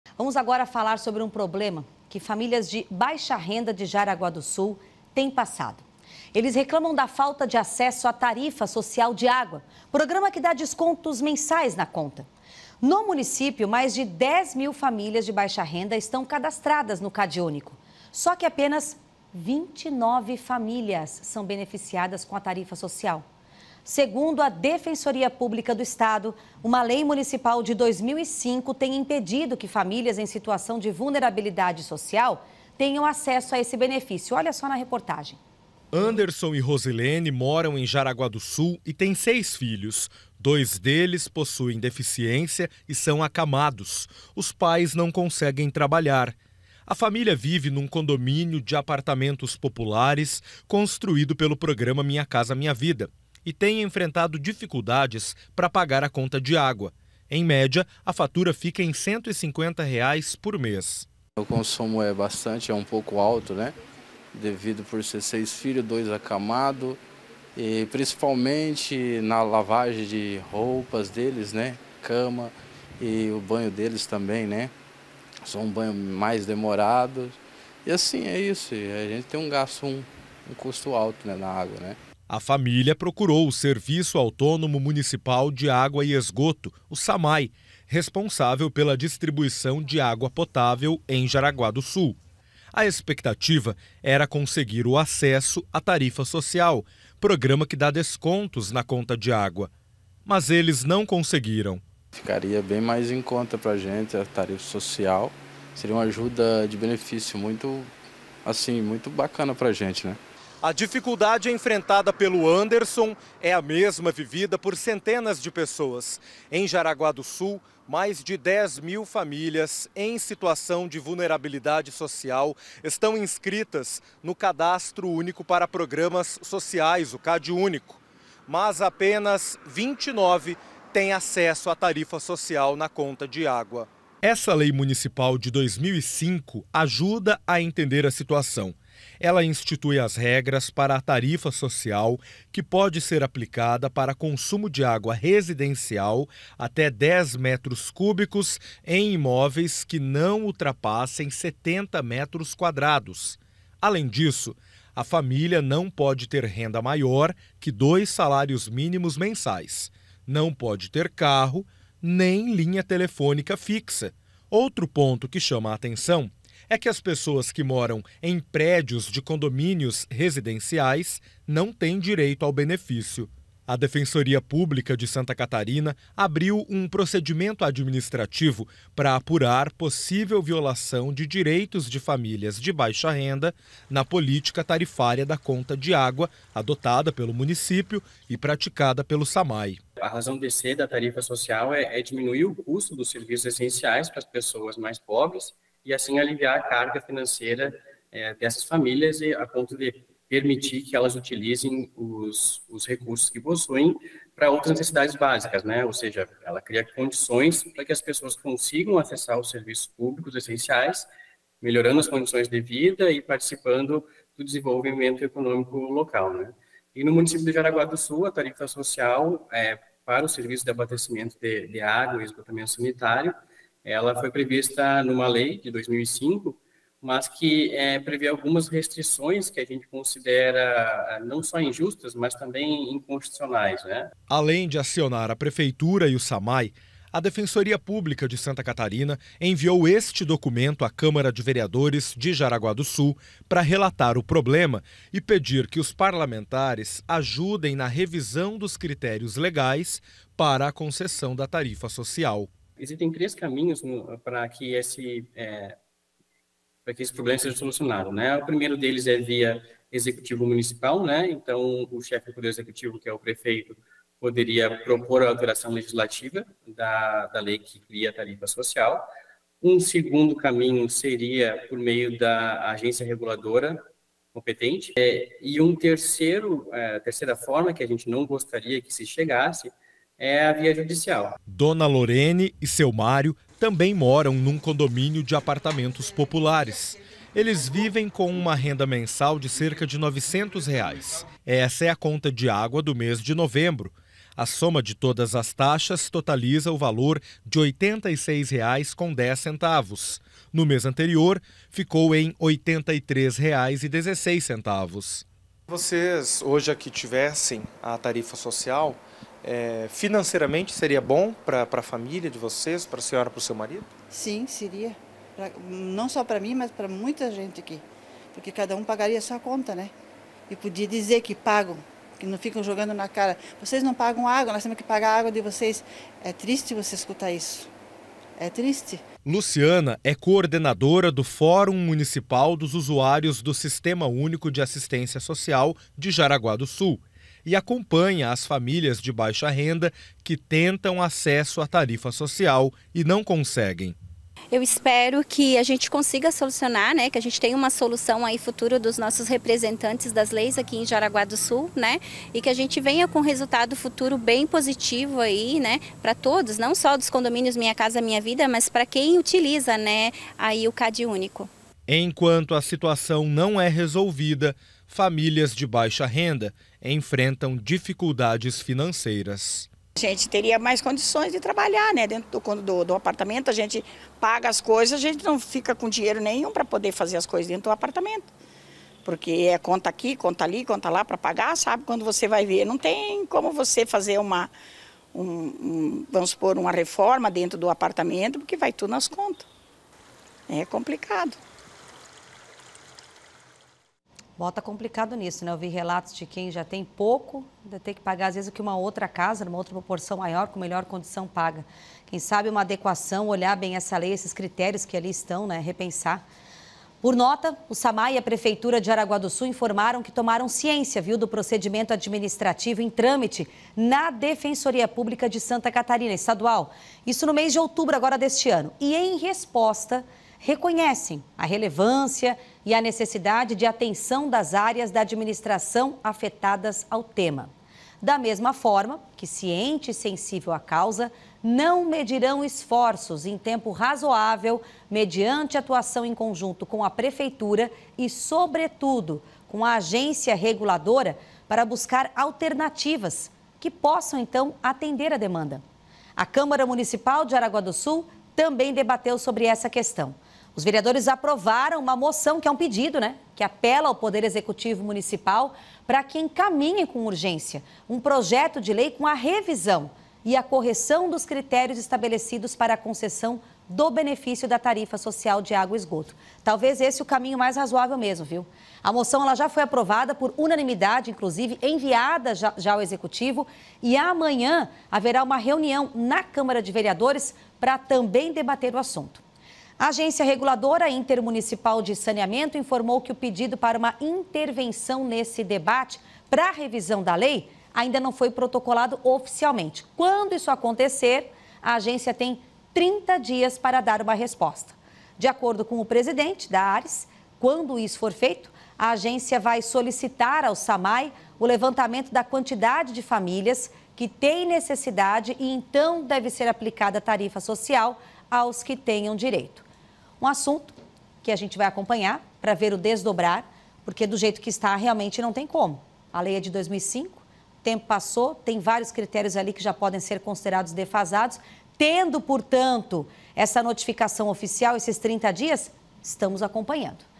Tarifa Social em Jaraguá do Sul: Famílias Buscam Soluções
A reportagem destaca as dificuldades enfrentadas por famílias de baixa renda em Jaraguá do Sul devido à falta de acesso à Tarifa Social de Água no município. Com depoimentos emocionantes, o vídeo explora os impactos sociais e financeiros dessa realidade, além de apresentar iniciativas locais e possíveis soluções para esse problema que afeta diretamente a qualidade de vida dessas comunidades.